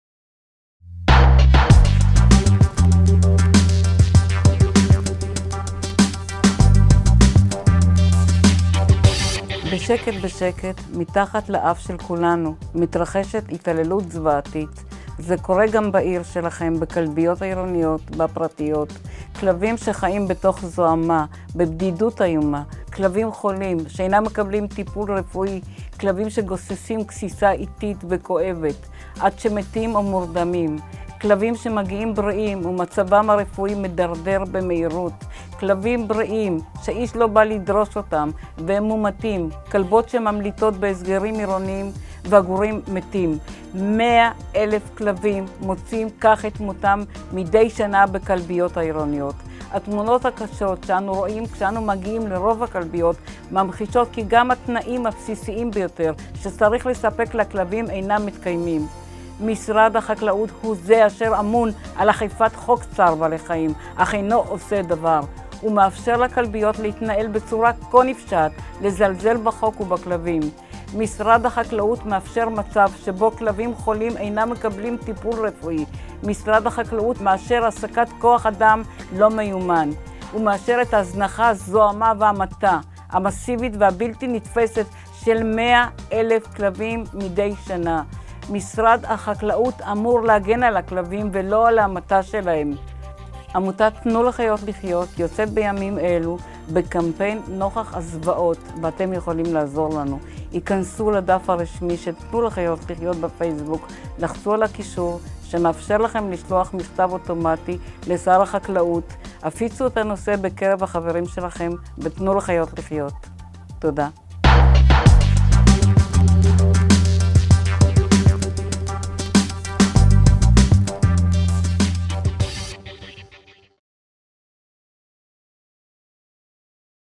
אתי אטלמן - התעללות זוועתית בכלביות העירוניות
מיקרופון לדמוקרטיה בטלוויזיה החברתית - בעלי מסר אקטואלי, מקבלים קול ובמה להעברת מסרים חדים לחיזוק הדמוקרטיה. הפעם, אתי אטלמן , מייסדת ודוברת עמותת "תנו לחיות לחיות, על ההתעללות הזוועתית בכלביות העירוניות והפרטיות וחוסר התנאים הבסיסי שאינו מסופק לכלבים. מצב הנגרם בגלל משרד החקלאות שאמון על הנושא אך אינו עושה דבר. לינק לשליחת מכתב לשר החקלאות